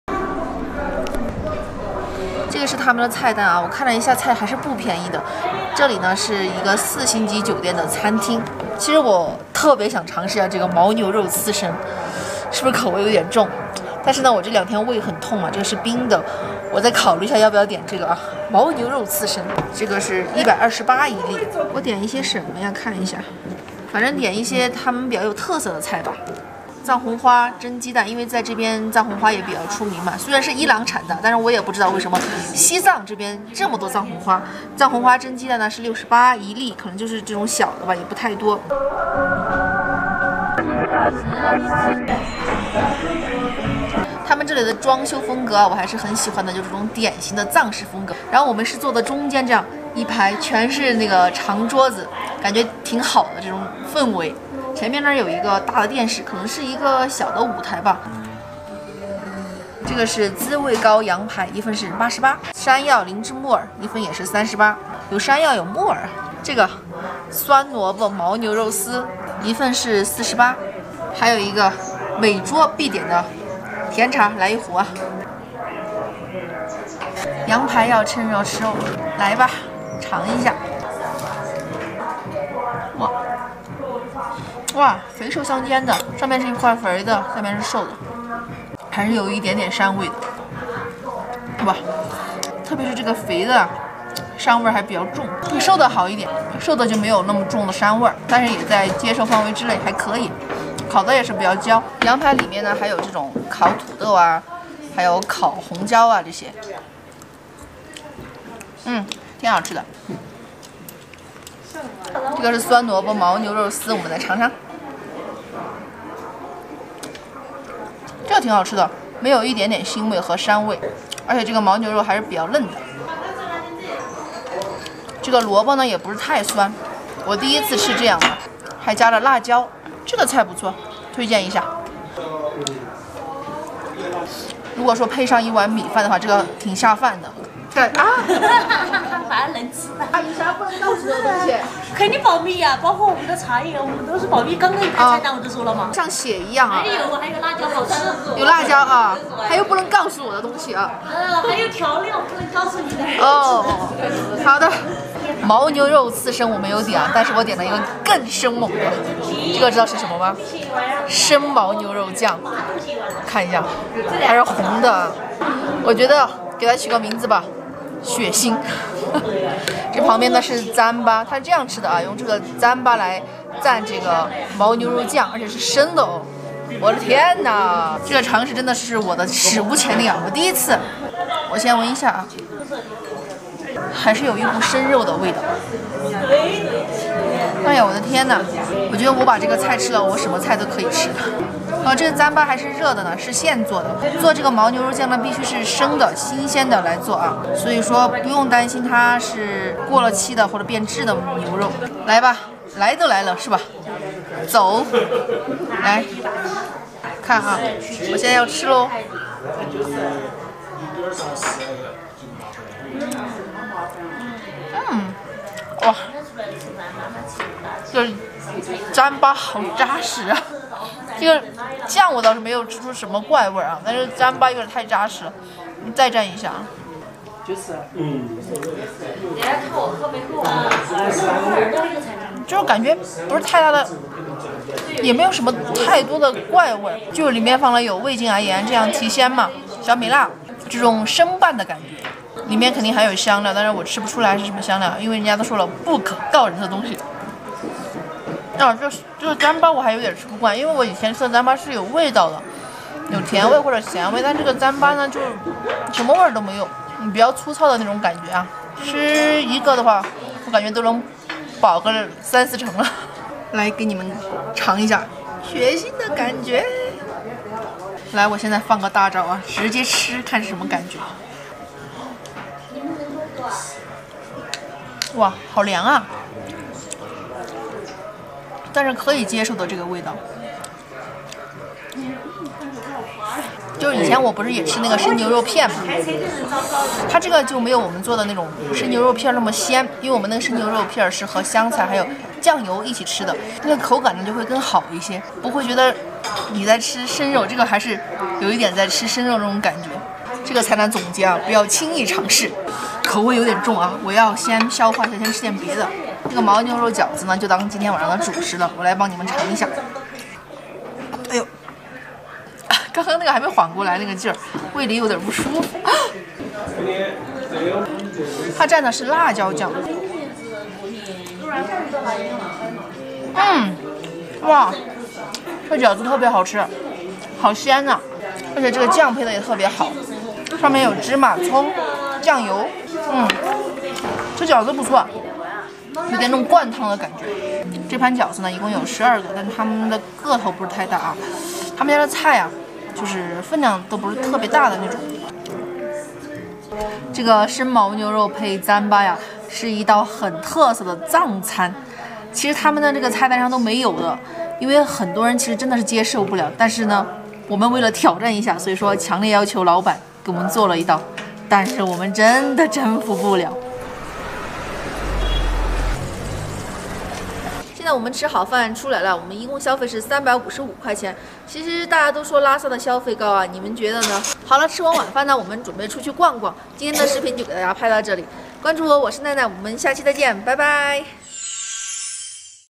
味道簡直了！生犛牛肉醬配糌粑，拉薩四星級酒店350RMB藏餐體驗 | 出發吧奈奈
味道簡直了！生犛牛肉醬配糌粑，拉薩四星級酒店350RMB藏餐體驗